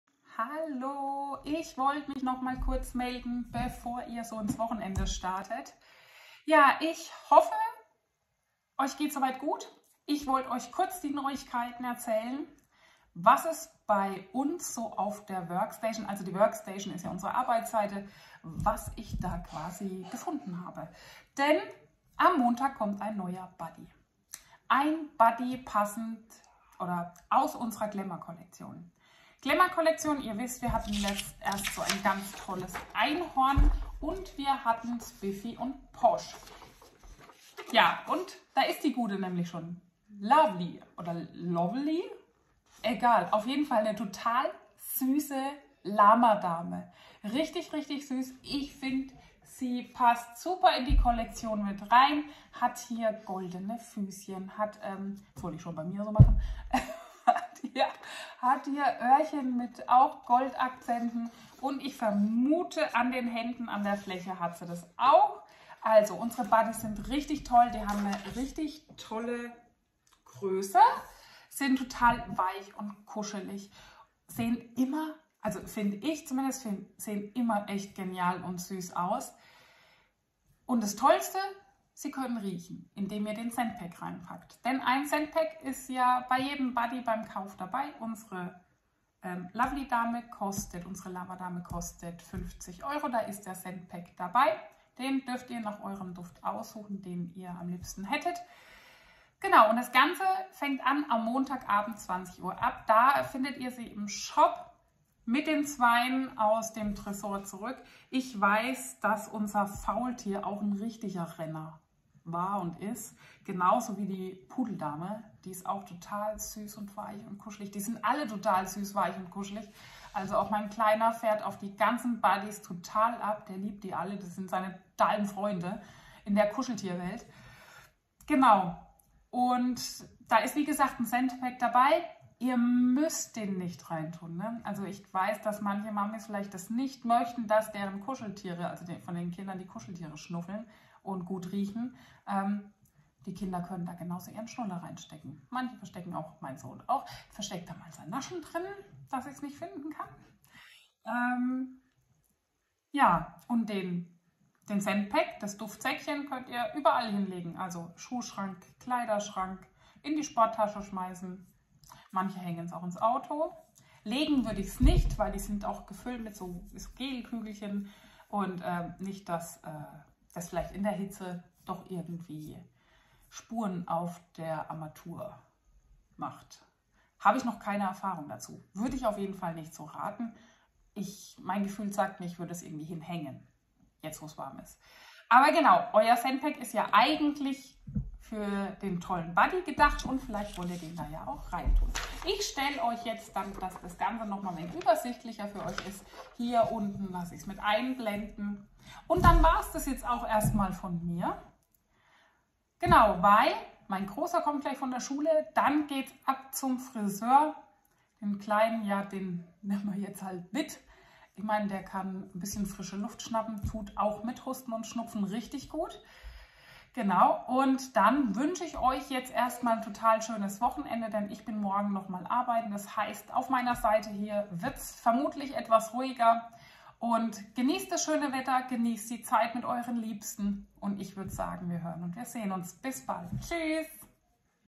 Es kommt ein neuer Buddy 🥰😍
Ab Montag kommt ein neuer Buddy. Eine zauberhafte Lama Dame. 🥰 Ich find sie mega süß und bin gespannt was ihr dazu sagt. ❤️ Habt ein schönes Wochenende